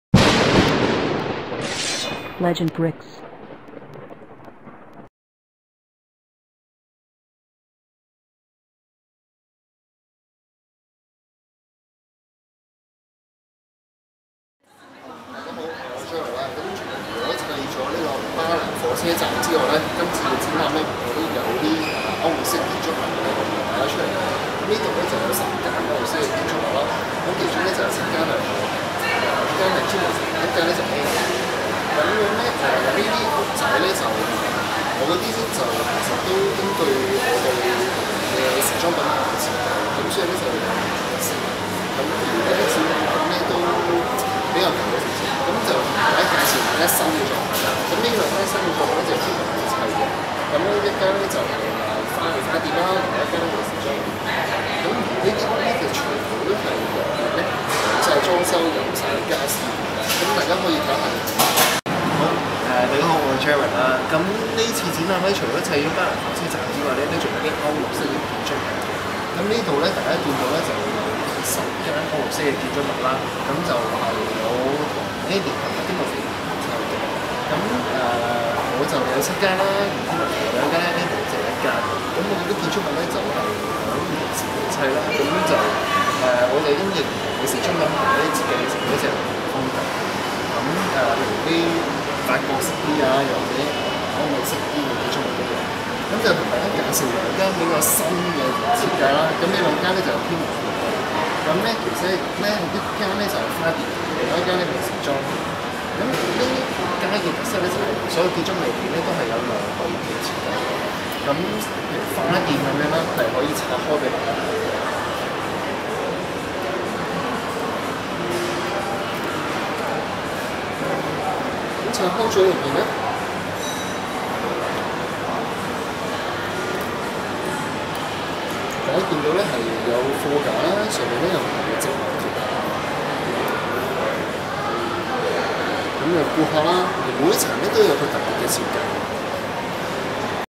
新元朗中心LEGO歐洲名店街介紹
新元朗中心LEGO尋樂作品展,作者Jared Chan介紹歐洲名店街作品.